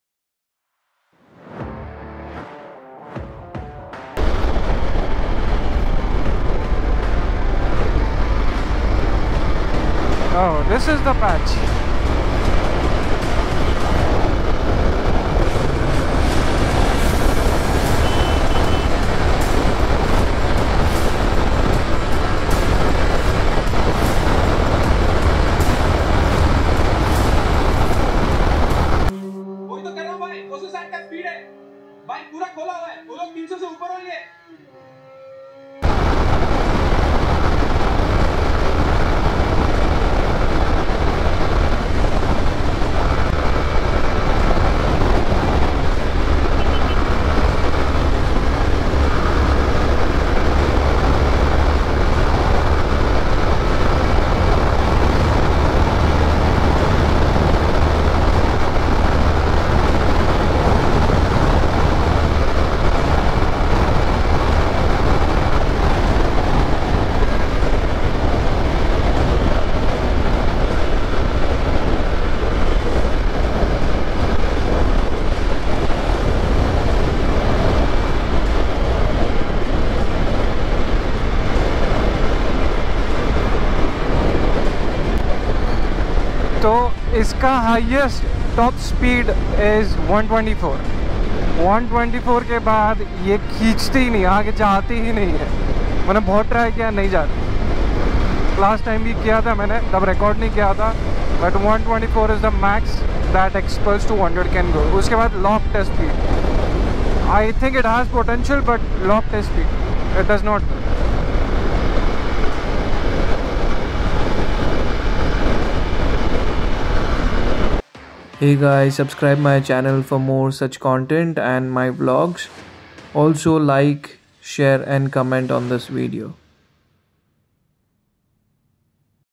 Xpulse 200 Top Speed on Highway // Pune to Goa, Banglore Highway
This is a small video which I have taken on my bike to check the top speed of it. I had pulled my bike's full throttle and the bike speeded till 124 km/hr. It is the maximum that Hero Xpulse 200 can go on a straight patch on a stock engine. If you make any modification it will definitely go more than that.

I hope this video was useful for the bike enthusiasts who are planning to buy Xpulse 200, according to my opinion if you are looking for a medium range off roader and a tourer then Hero Xpulse 200 is a very good option in the market. I will be soon uploading my Pune to Goa vlog in which you will be able to see how easily this bike has tackled the worst condition roads. I have cruised at the speed of 90 km/hr to 110 km/hr which was sufficient to cover distance in good short span of time.